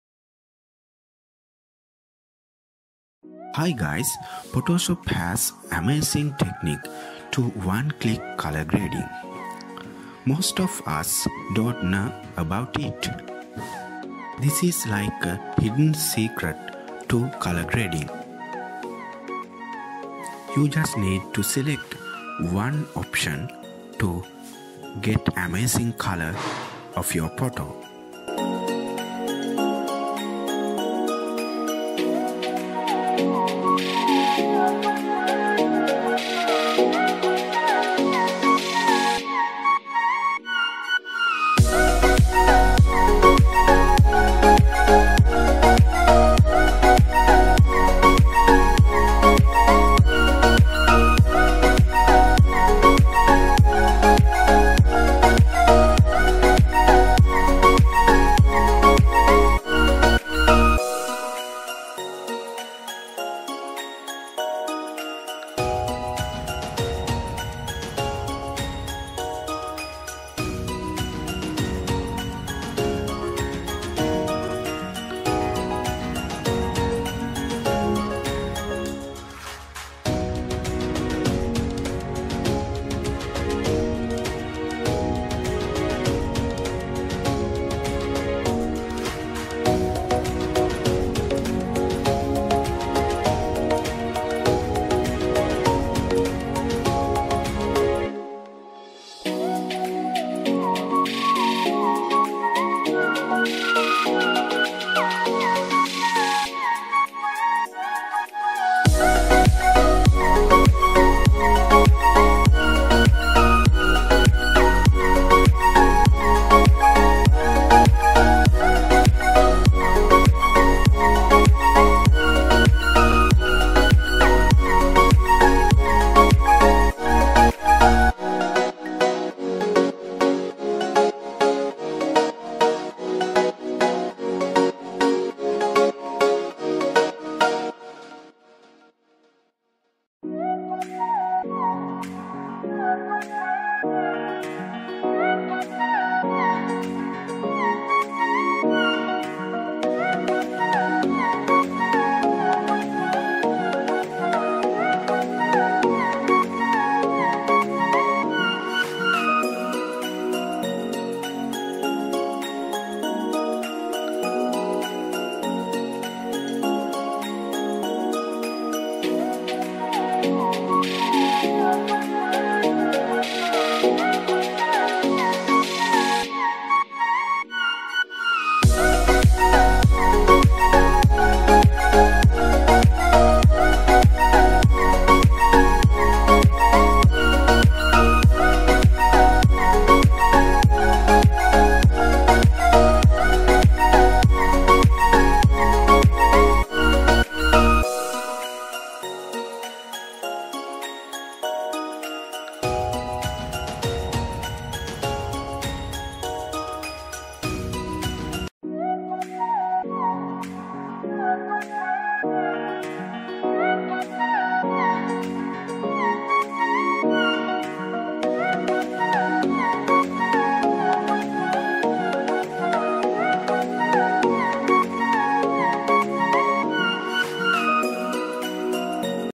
One Click Color Grading - Photoshop Has Hidden Secret To Quick Color Grading In Photoshop
Simple fast speed color grading trick
Postprocessing color-grading is super easy with this secret technique.

No more sliders. No need presets, Actions, adjustments. Photoshop has this super amazing one-click technique to make 4 color grading tones.